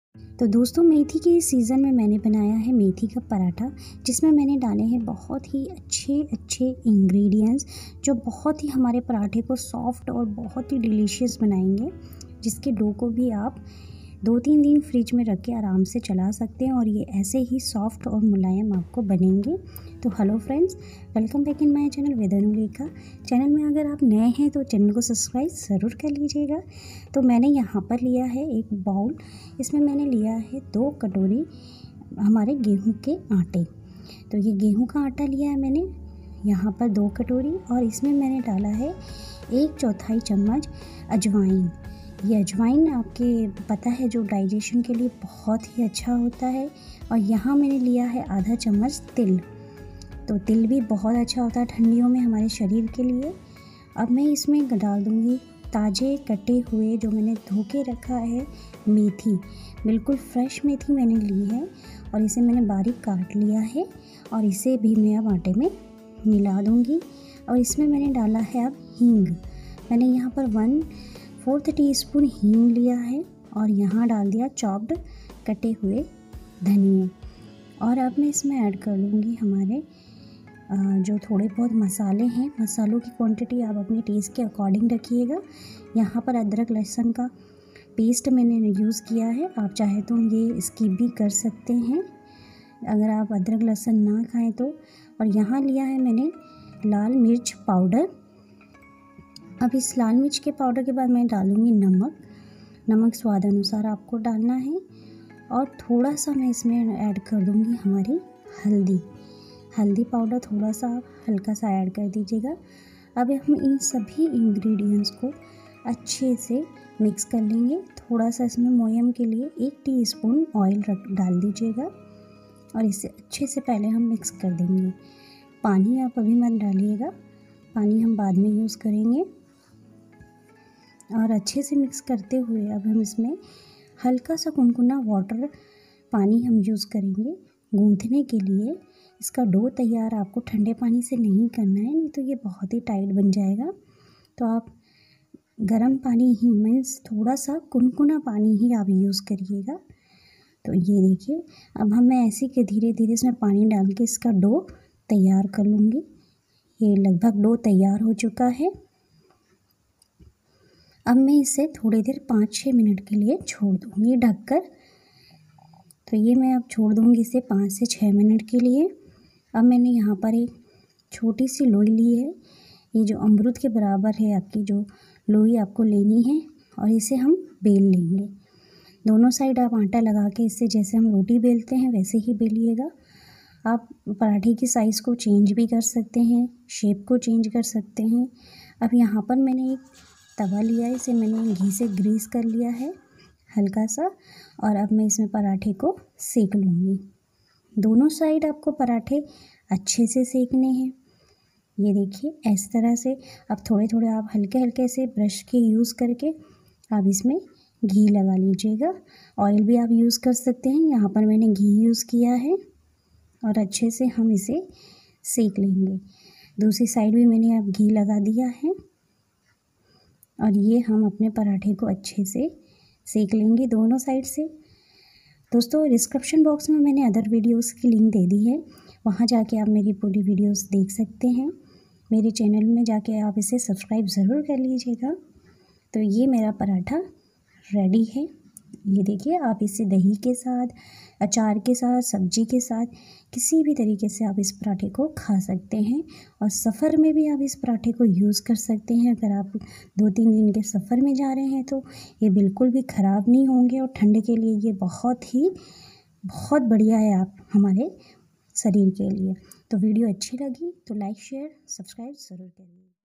सर्दी में खाए जाने वाले मेथी के पोष्टिक पराठे secret ingredients के साथ#With Anulekha Singh
Hello dosto

With Anulekha Singh Vlogs
Aj mane share kiya h methi paratha with some secret ingredients

Sweets of India

chatpata masala food

Mix content vedioes

Beauty of every woman

Immunity booster and healthy food

Kids zone

भारतीय आस्था